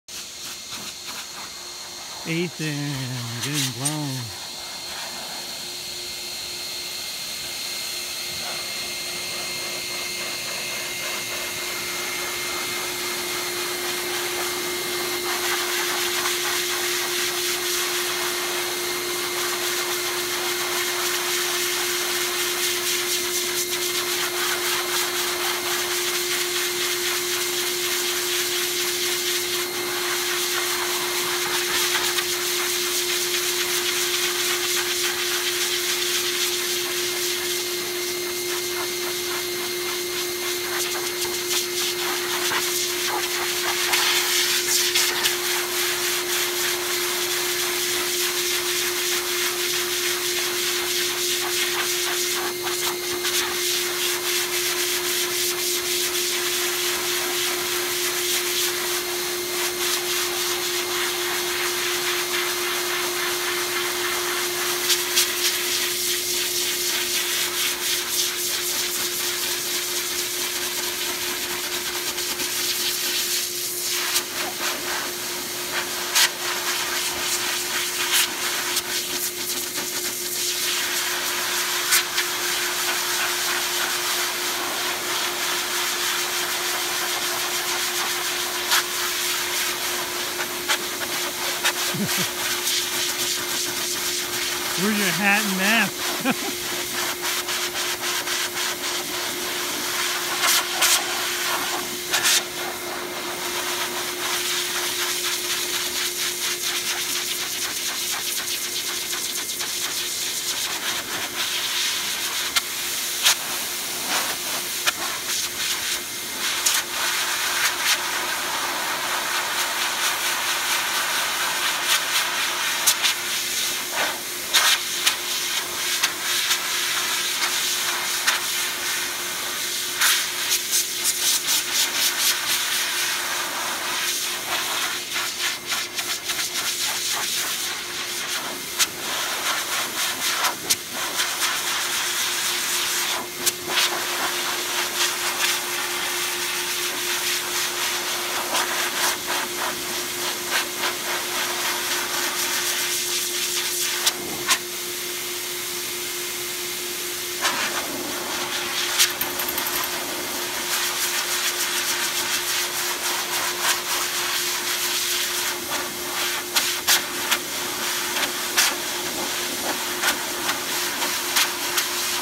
Siberian Husky Coat Blowing - Grooming a siberian husky -Shedding
Aethon - Ninkasi's Rising Sun getting deshed done with a Metro Vac Master Blaster 8 HP blower